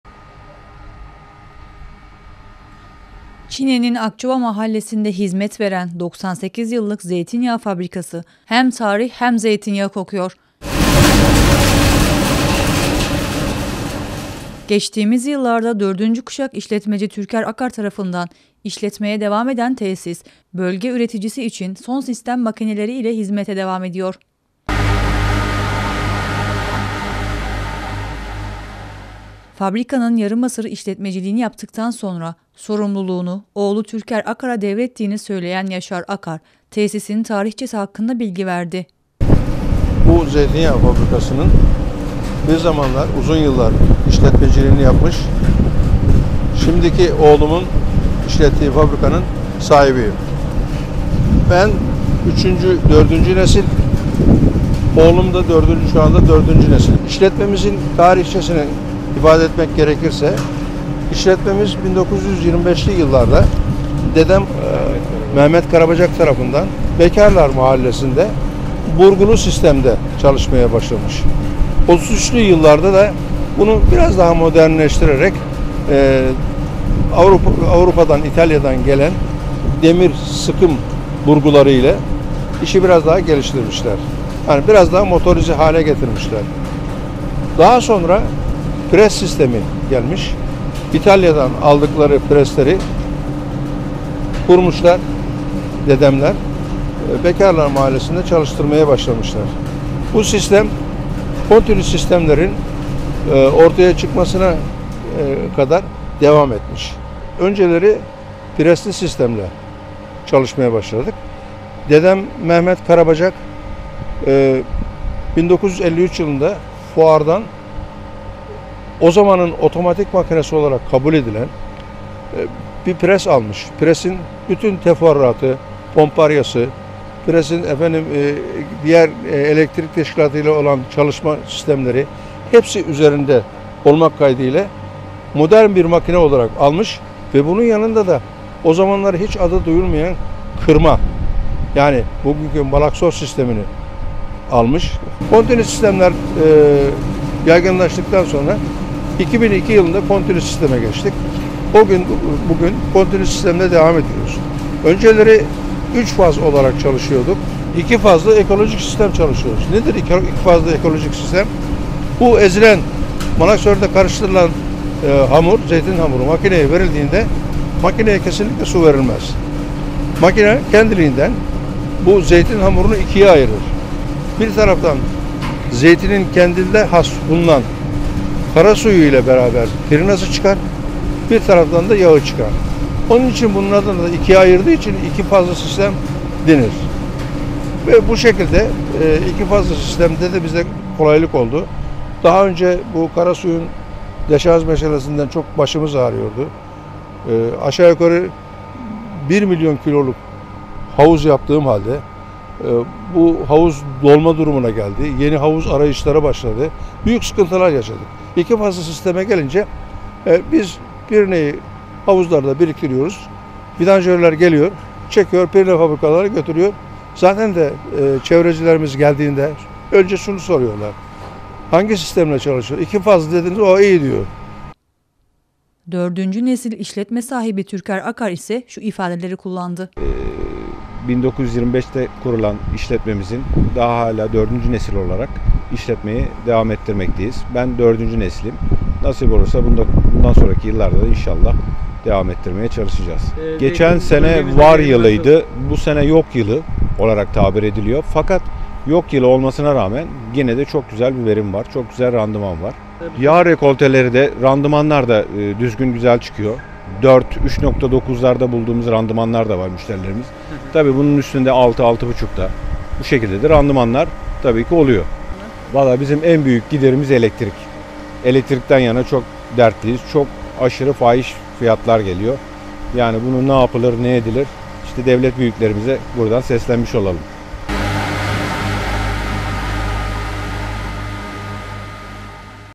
Asırlık tesis modernize olarak hizmete devam ediyor
Çine’de 1925 yılında Mehmet Karabacak tarafından kurulan zeytinyağı fabrikası, üçüncü kuşak 78 yaşındaki Yaşar Akar tarafından 2002 yılında modernize hale getirilirken, dördüncü kuşak Türker Akar tarafından hizmet vermeye devam ediyor.